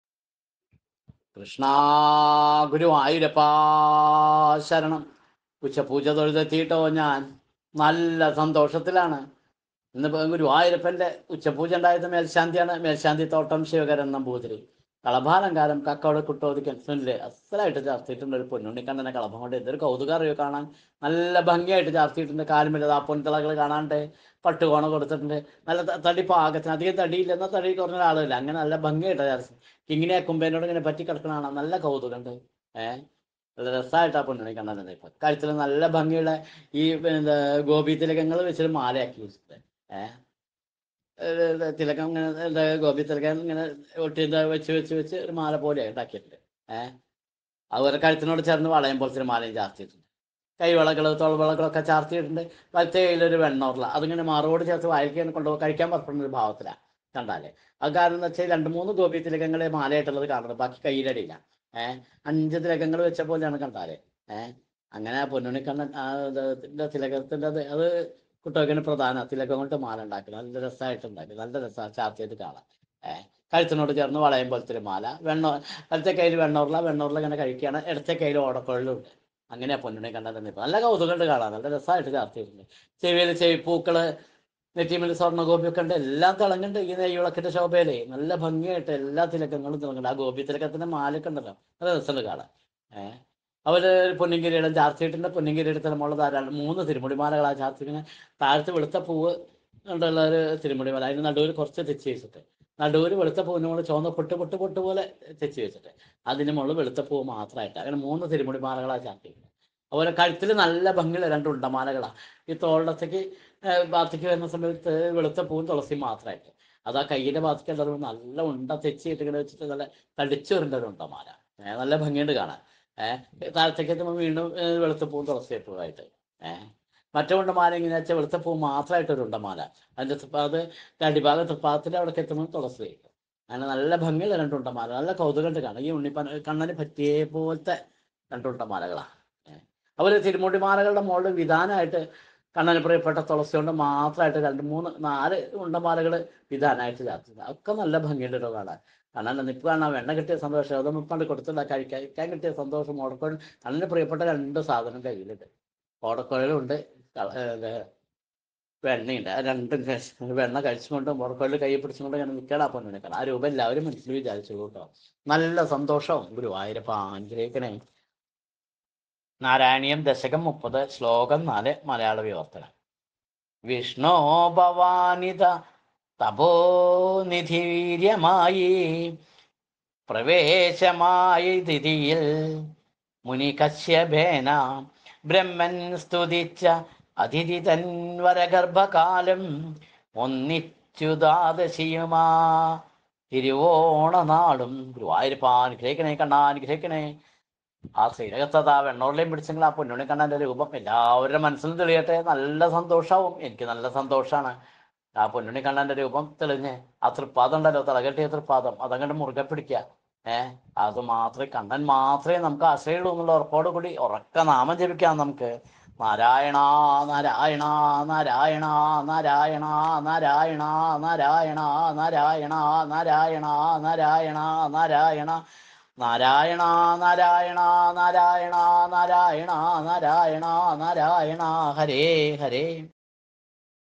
ഗുരുവായൂരപ്പന്റെ ഉച്ചപൂജ അലങ്കാരവർണ്ണന | 07 May 2023 | Guruvayurappan Uchapuja Alankaram
ഗുരുവായൂരപ്പന്റെ ഉച്ചപൂജ അലങ്കാരവർണ്ണന ഗുരുവായൂർ ക്ഷേത്രം
Hare! Guruvayurappa!